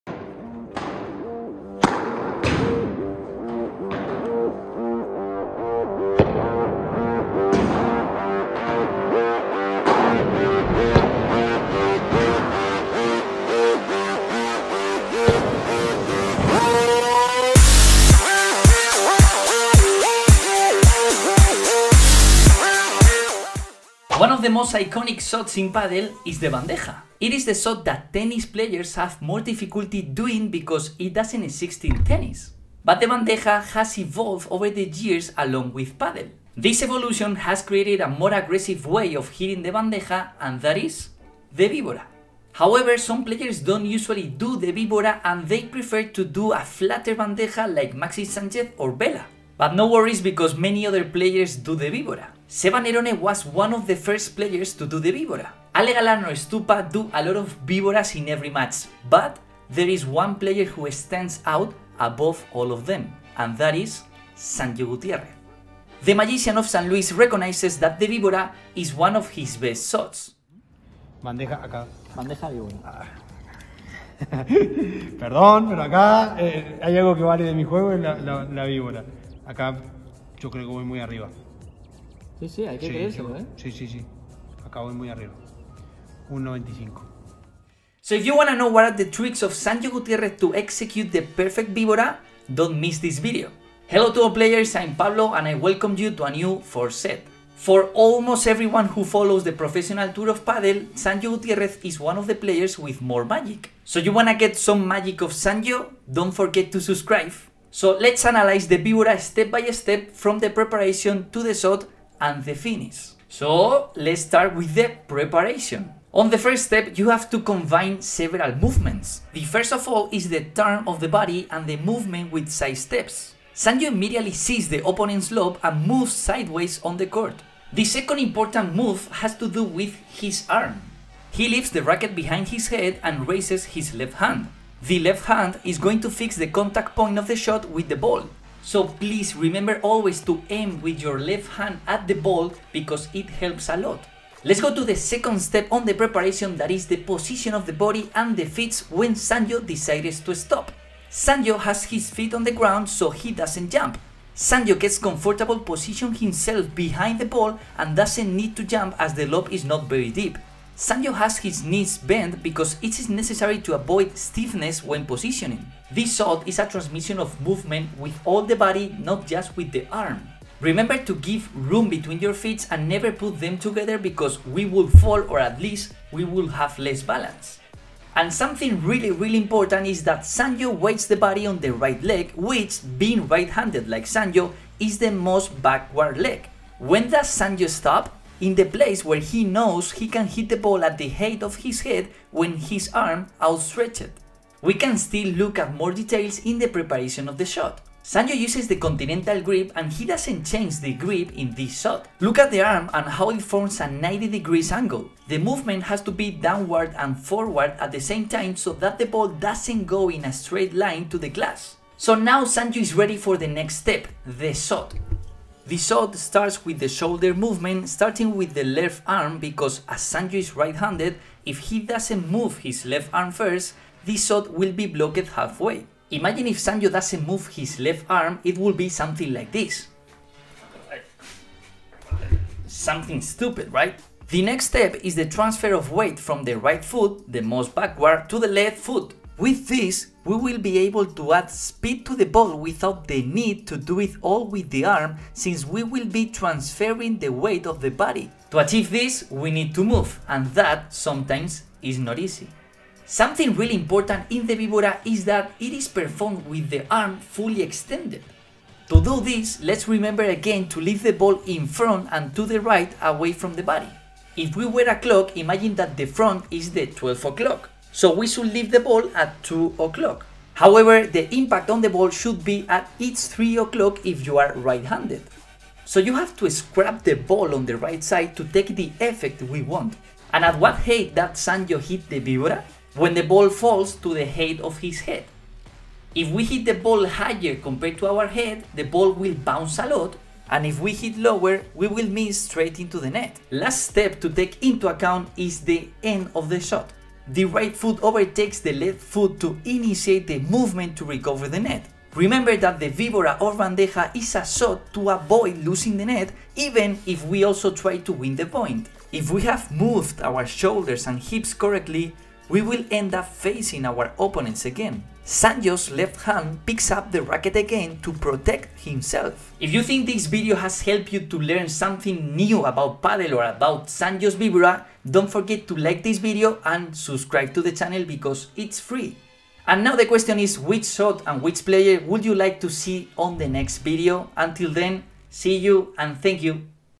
IMPROVE YOUR VIBORA IN 5 MINUTES ft SANYO *PADEL SMASH* - the4Set
Improve your vibora in padel in 5 minutes with Sanyo Gutierrez. Sanyo is the most magical player of the World Padel Tour and his vibora is one of the best. But, how he does it? What are his tricks? Learn from the best!